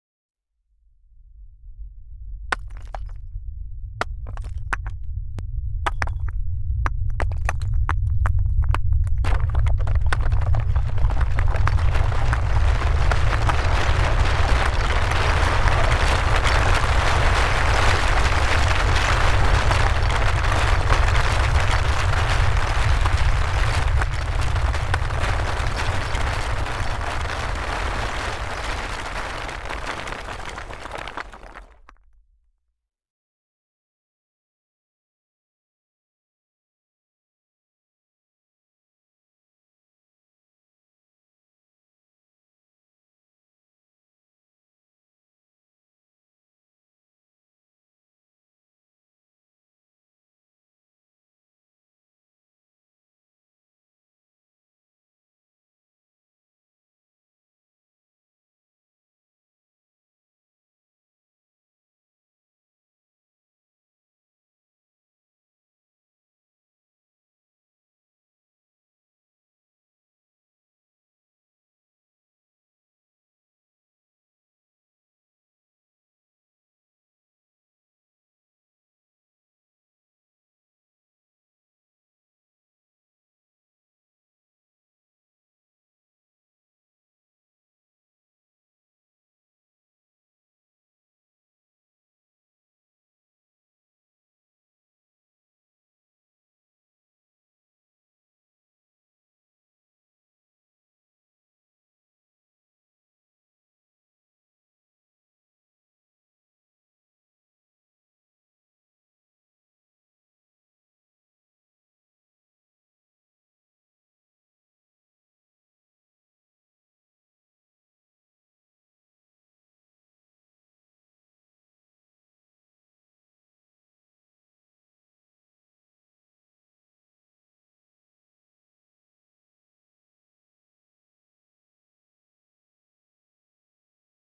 End of Day is near 2022
End of Day is near please ALLAH KAREEM ki rassi mazboti sy tham len. ALLAH KAREEM hum sub ka khatma Eiman per kary. Aamin